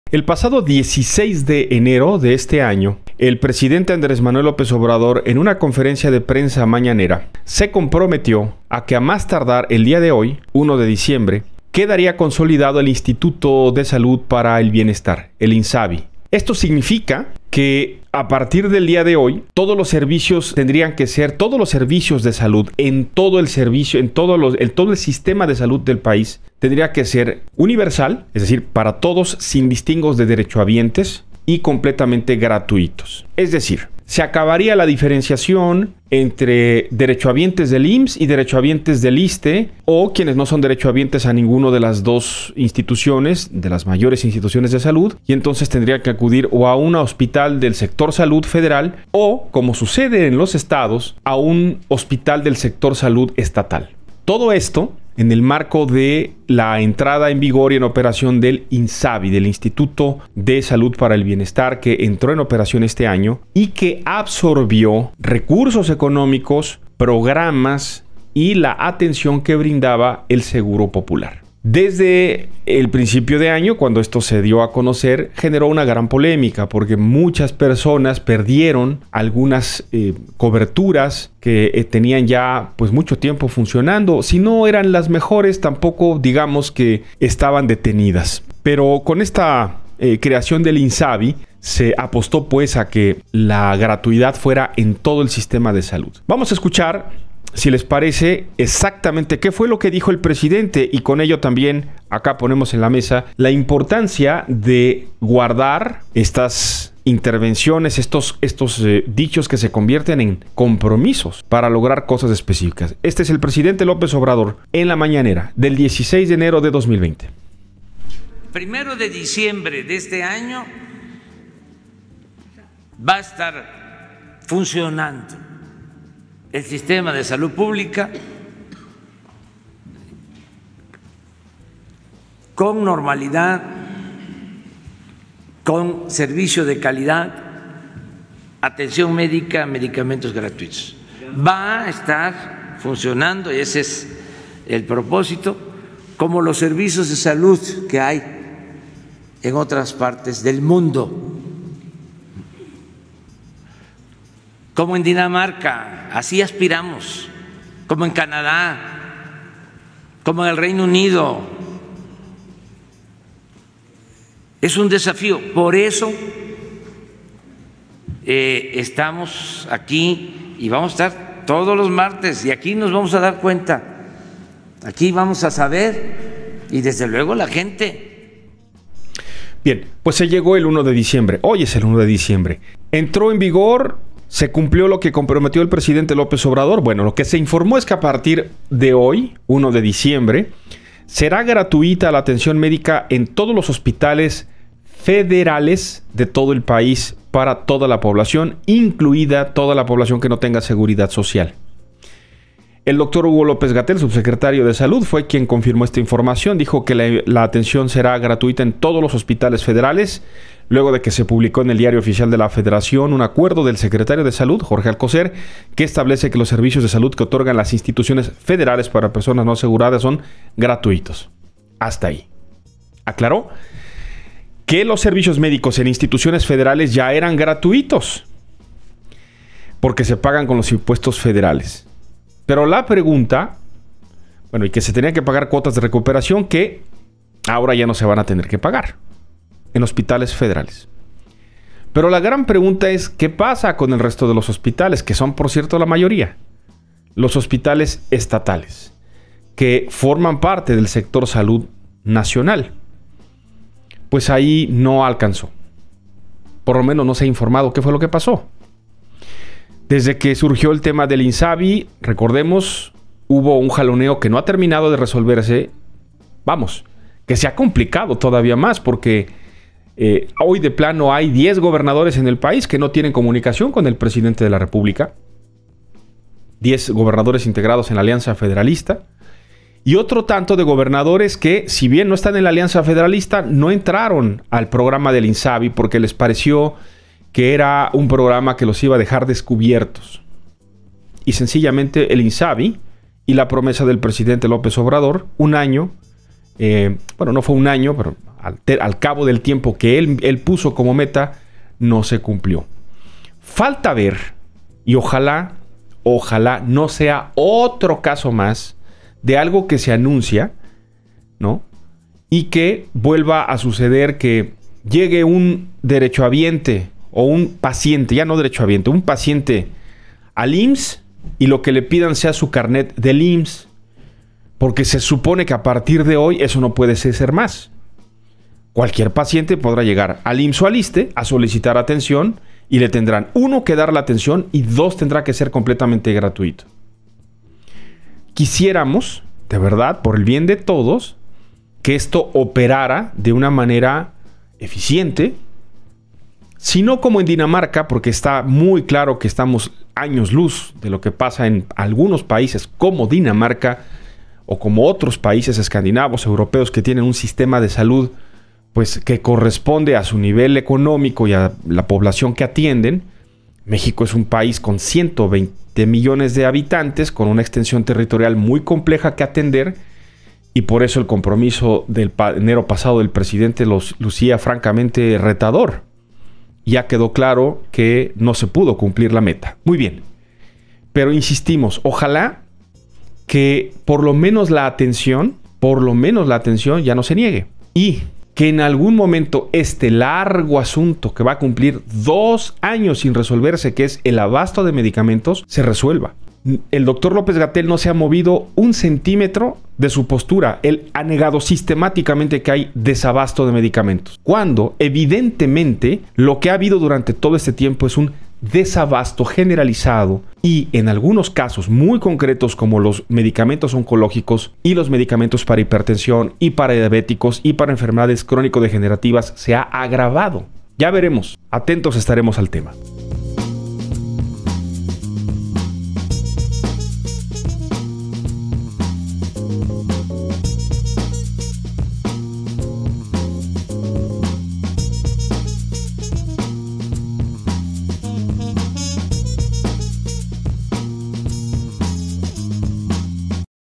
¿Ya tenemos un sistema de salud como el de Dinamarca?
A partir del martes 1 de diciembre será gratuita la atención médica en los hospitales federales de todo el país para toda la población, incluida la que no tenga seguridad social, informó el subsecretario de Salud, Hugo López Gatell

¿Dónde pueden encontrarnos?